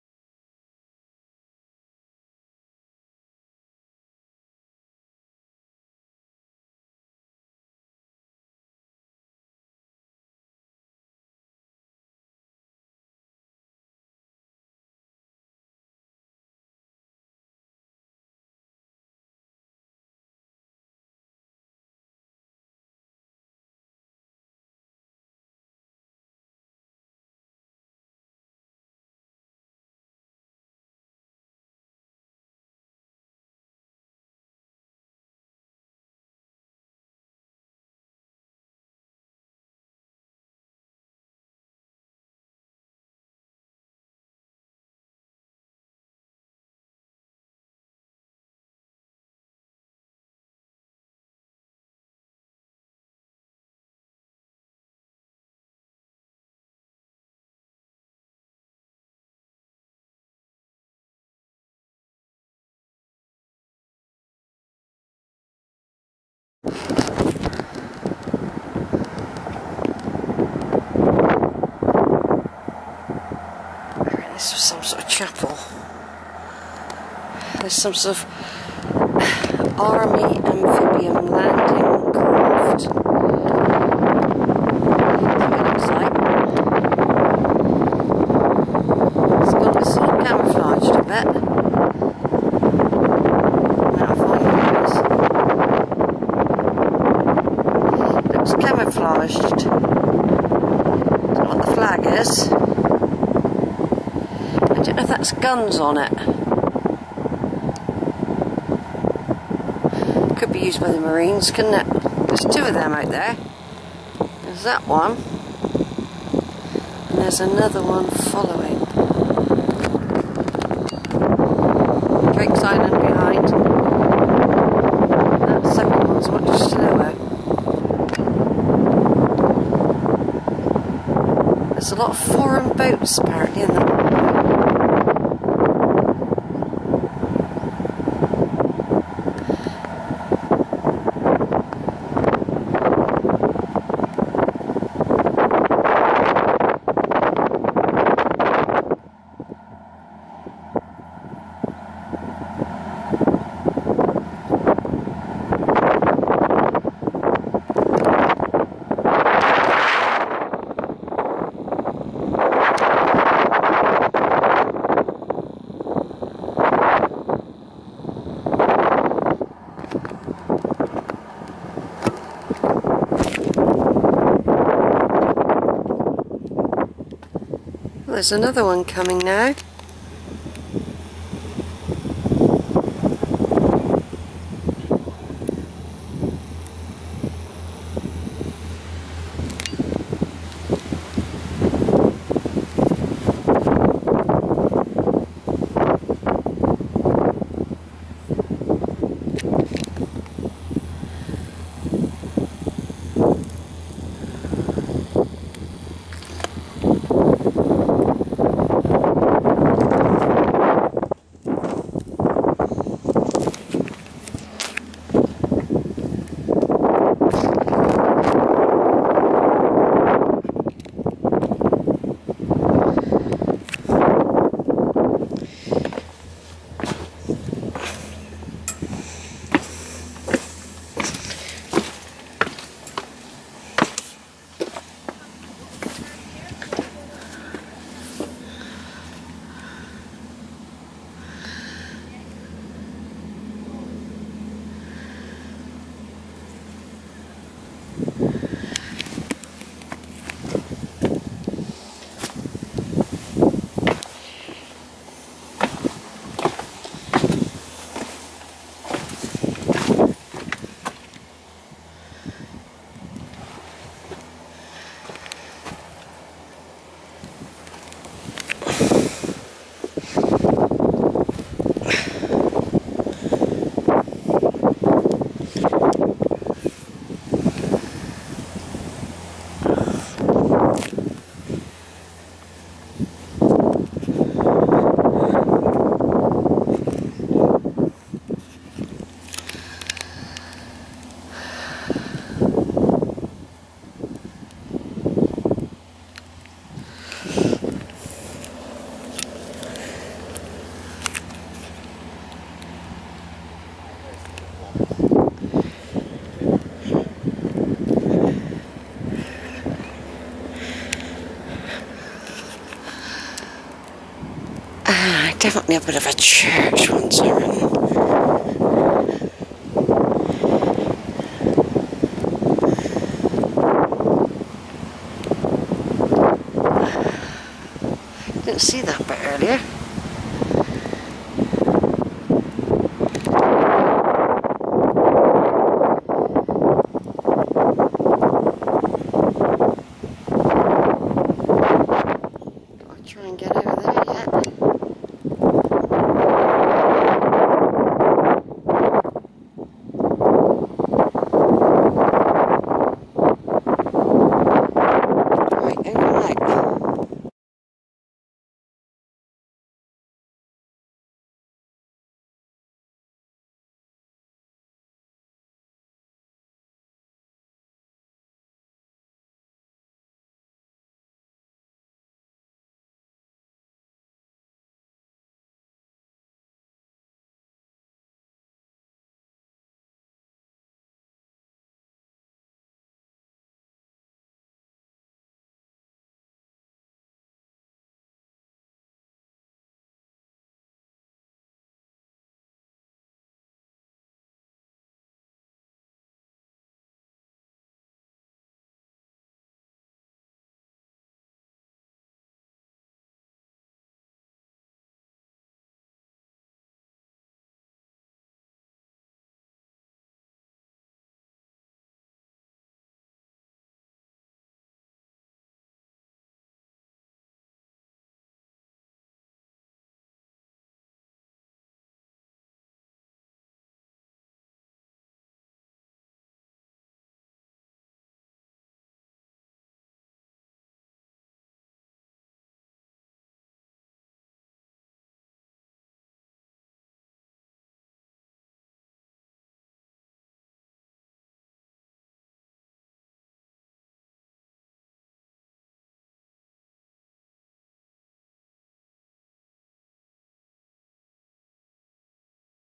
Cawsand & Rame Head And Mt Edgecumbe Park Visit, Part XIII, ' Fake Ruin Folly' by Sheila, Sept 12th
This folly is a fake ruin which replaced a navigation obelisk. Built in 1747 from Medieval stone from the churches of St George and St Lawrence, Stonehouse , Plymouth. I climb up inside the folly to get some good views of the River Tamar, Drakes Island and various marine craft.Over the past couple of years the Royal Marines have moved from Poole in Dorset to Devonport where there is now a specialist training centre for 'Landing Craft Training'--amphibious training at RM Tamar. Assault squadrons of the RM are assigned to the ships Ocean, Bulwark and Albion---The large camouflaged landing craft I observed are in fact the Large land Craft Utilities vehicles capable of carrying a Challenger 2 Tank or 5 RM Viking armour vehicles it is quite likely that the RM were training today when I spotted them.